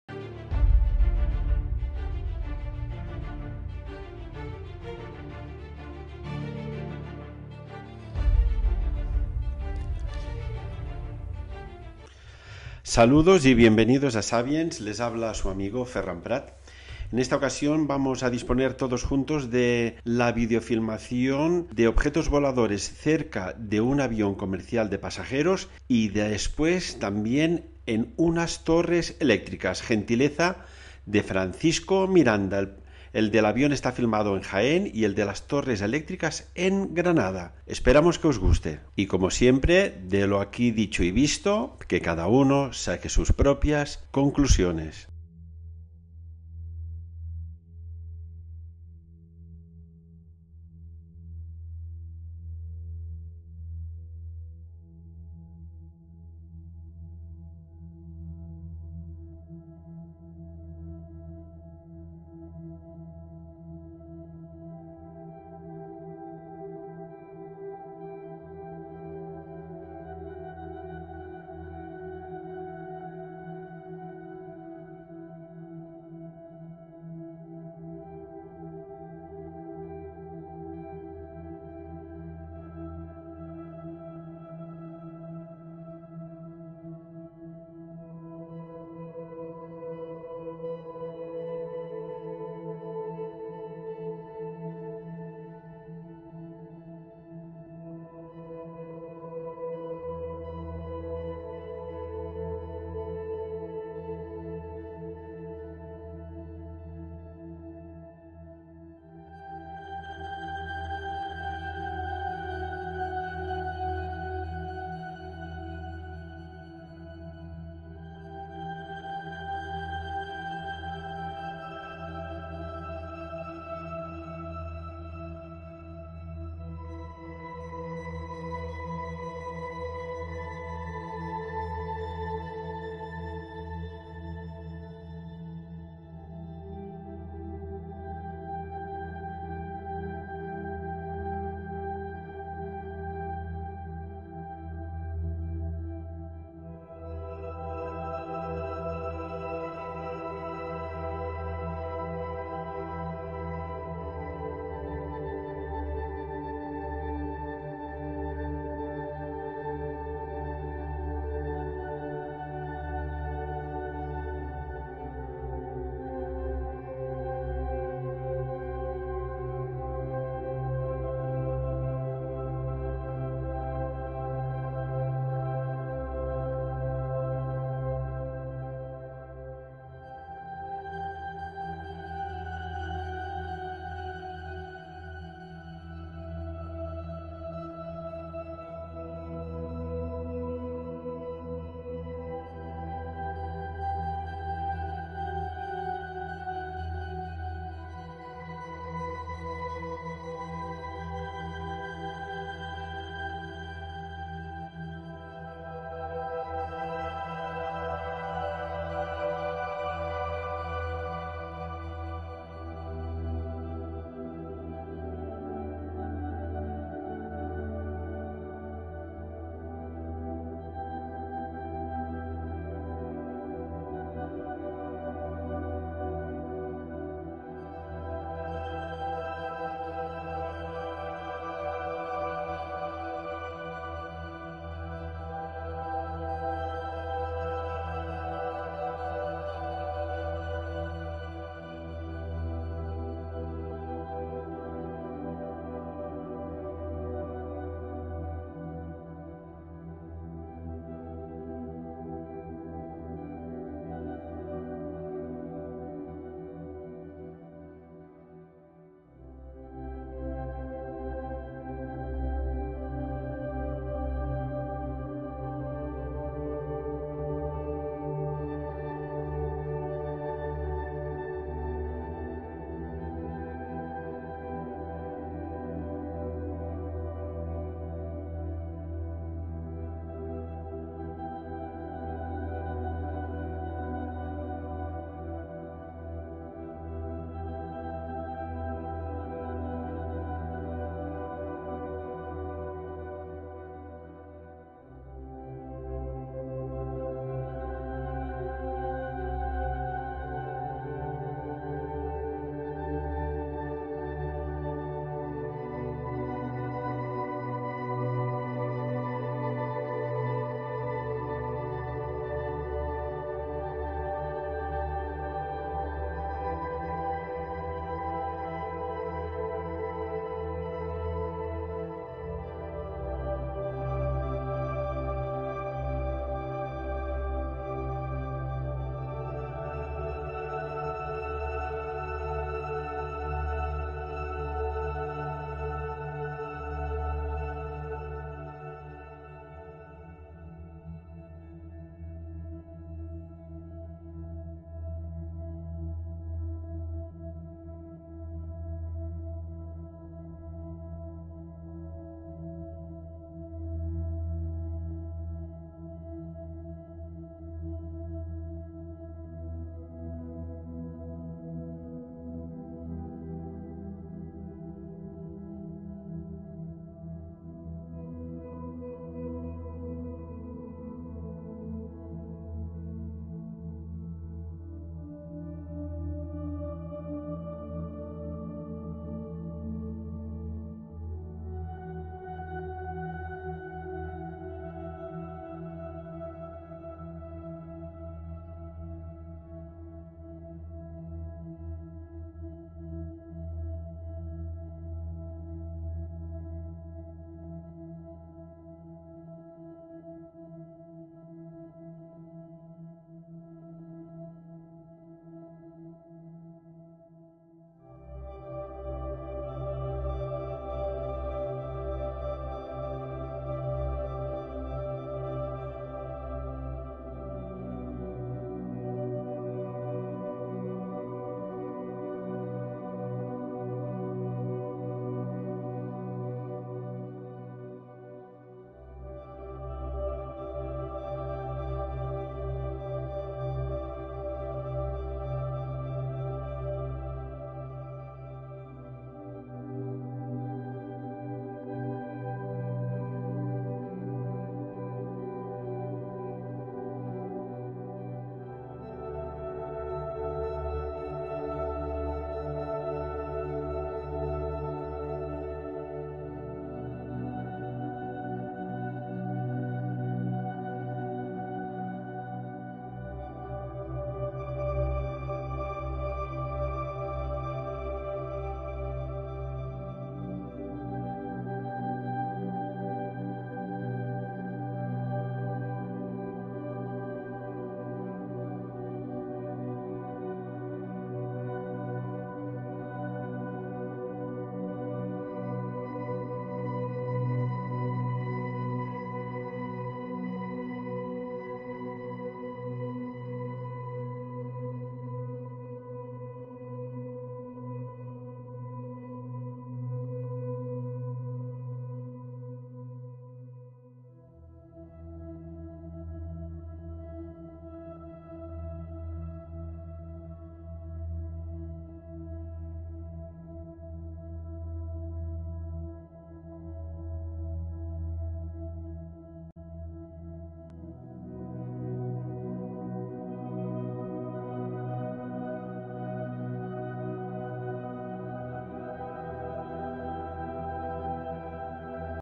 Filmados OVNIS-UFO cerca de avión de pasajeros y en Torres Eléctricas
Agradecemos vuestra colaboración en la aportación de vídeo filmaciones de la presencia de Objetos Voladores No Identificados. En esta ocasión nos los reporta Francisco Miranda que filmó a finales del año 2018 y en febrero del 2020 dos vídeos de OVNIS, Uno captado en Jaén y el otro en Granada,

Agradecemos su colaboración y aportación a Sabiens de estos dos interesantes documentos visuales. Esperamos que os guste.

Sabiens OVNI-UFO